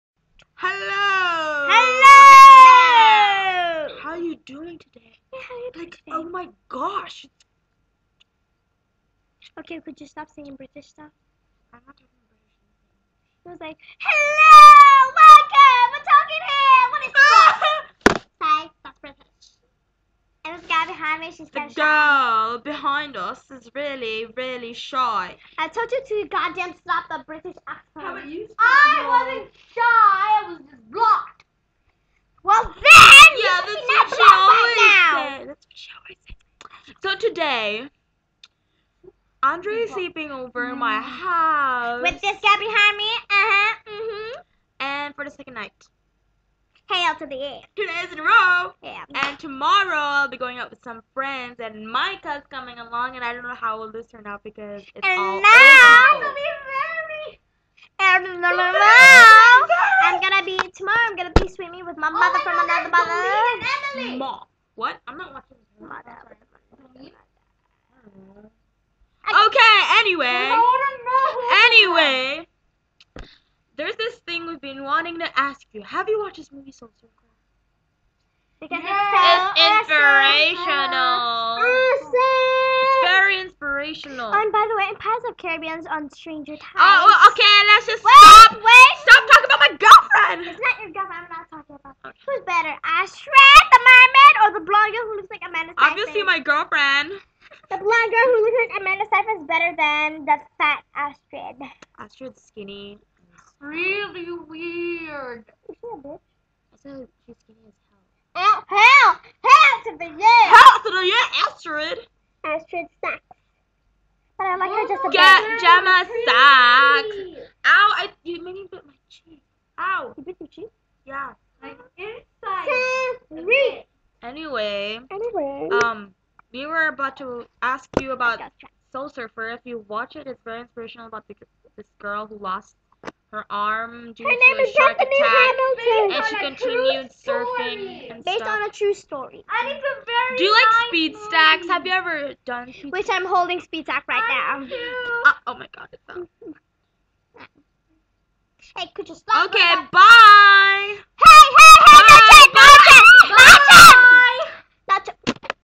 Just Another Vlog
Andre and Micah at a sleepover at mine